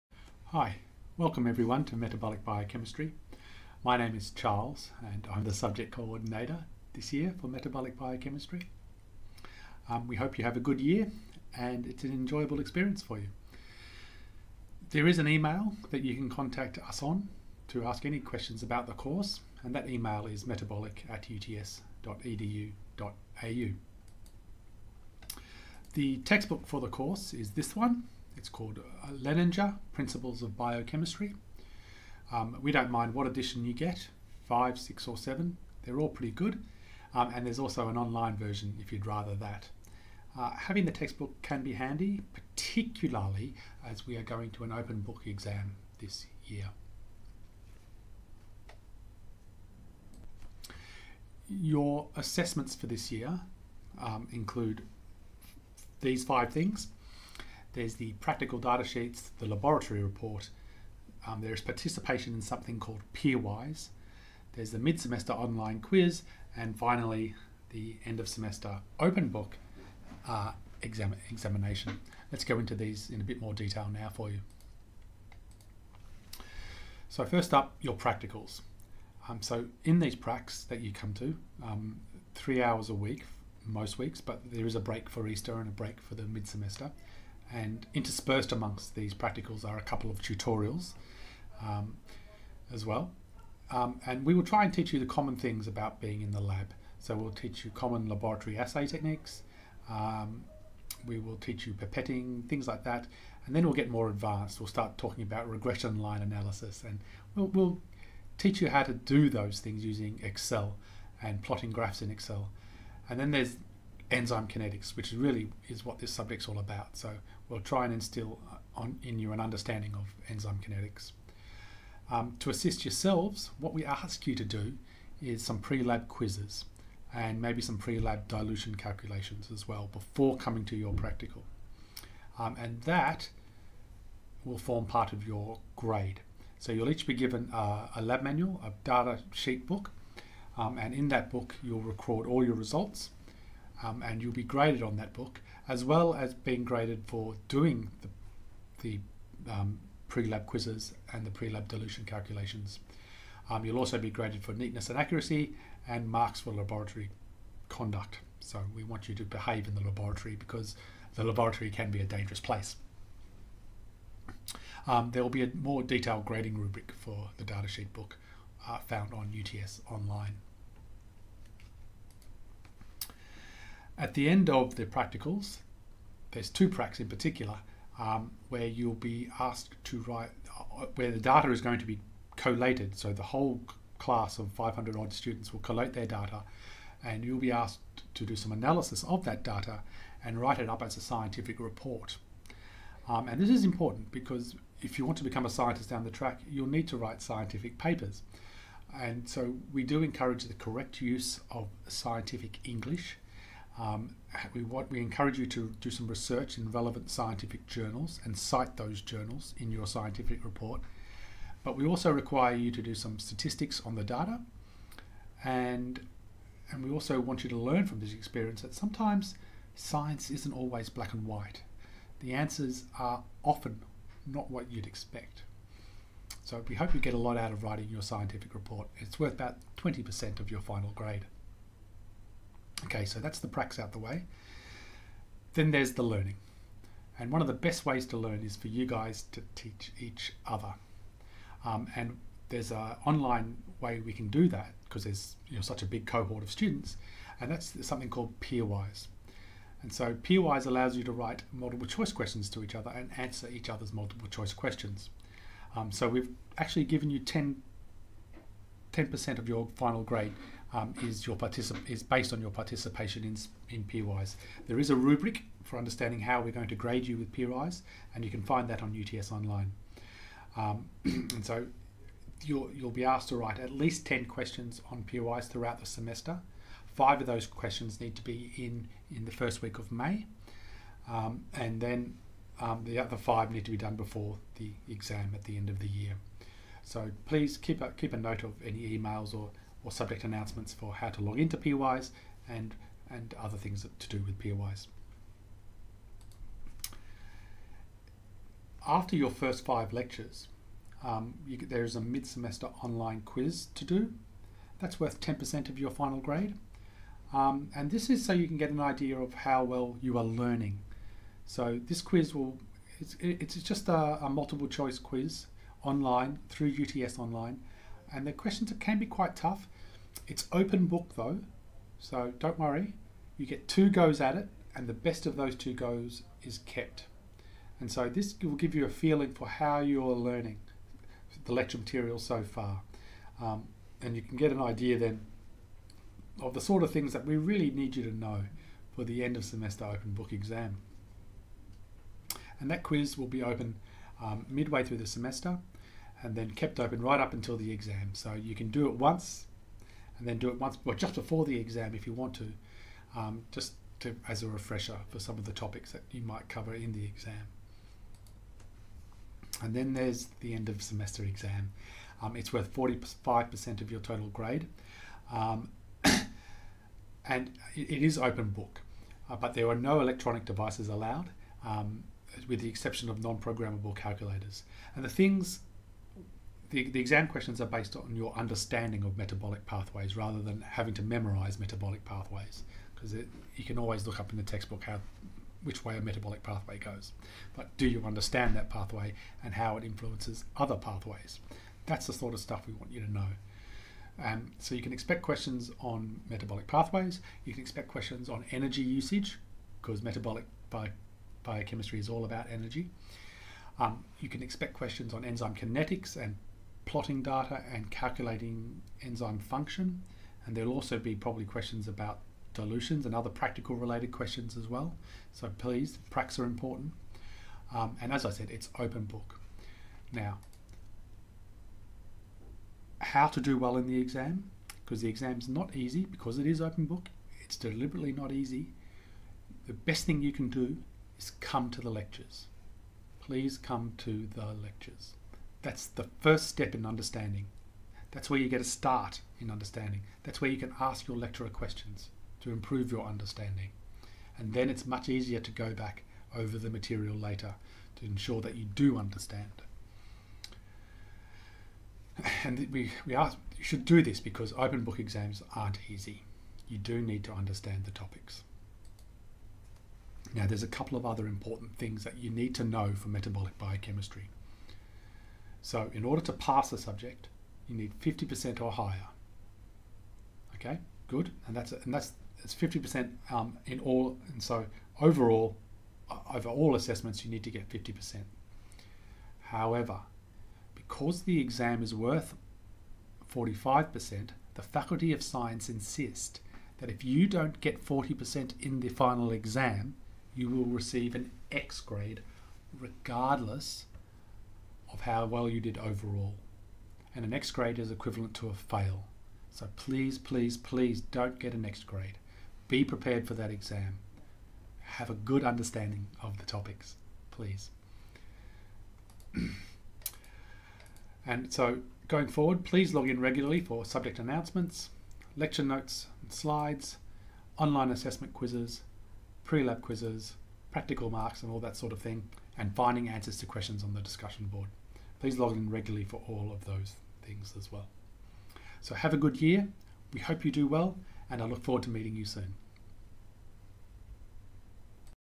Intro to 91320 Metabolic Biochemistry 2018
A brief intro on how students will be graded in 91320 Metabolic Biochemistry in 2018